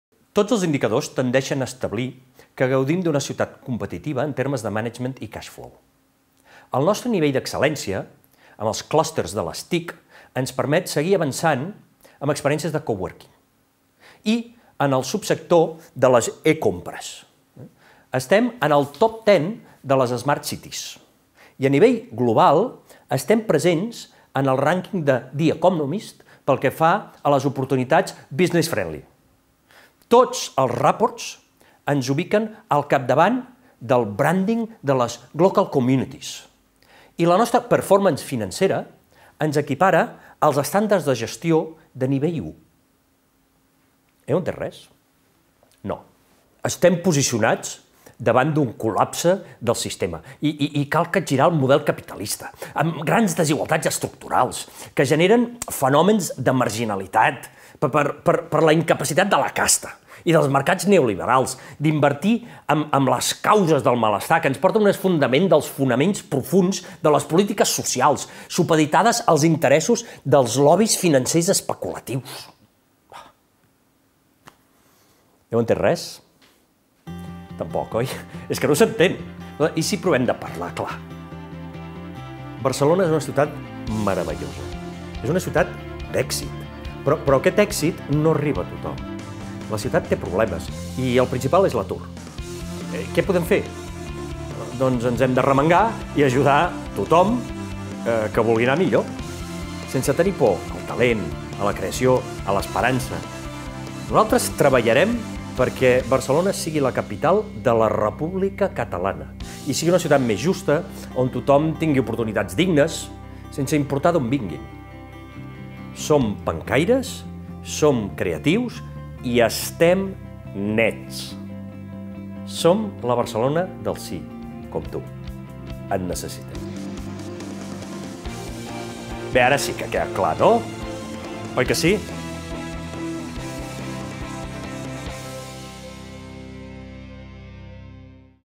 Bosch parodiant el discurs de Trias i Colau
El candidat d'ERC contraposa el discurs dels altres candidats amb el seu lema, 'És l'hora de parlar clar'

El candidat d'ERC a l'ajuntament de Barcelona, Alfred Bosch, ha fet públic un nou vídeo de campanya, titulat 'És l’hora de parlar clar', en què parodia els discursos de Xavier Trias i d'Ada Colau, imitant-los, volent fer veure que els tecnicismes amb què parlen els uns i els altres no és pas gens pròxim a la ciutadania